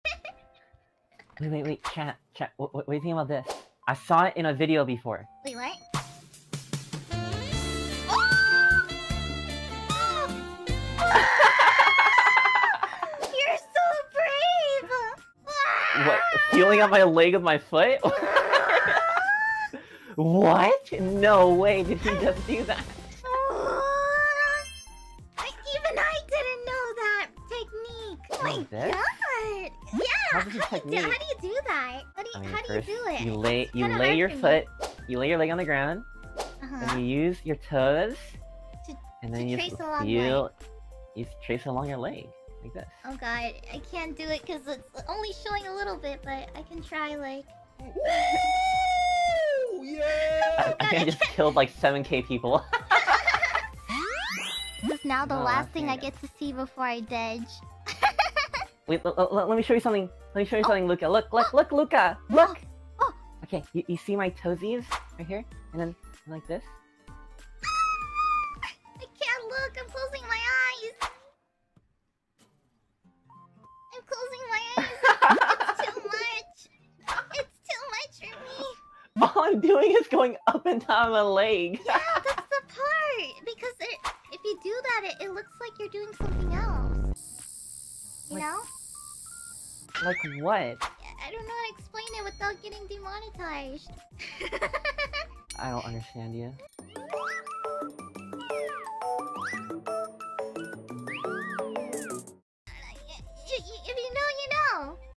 finana gets flustered by aster's leg trick, shows it to luca after 【 NIJISANJI EN | Aster Arcadia 】
my oshi has such nice legs i'm jealous omg

💫 timestamps
0:00 start
0:06 the leg thing
0:31 aster teaches feesh
0:55 luca in chat
0:59 aster shows luca
1:24 ayo it looks like what?

twt: @ziemawari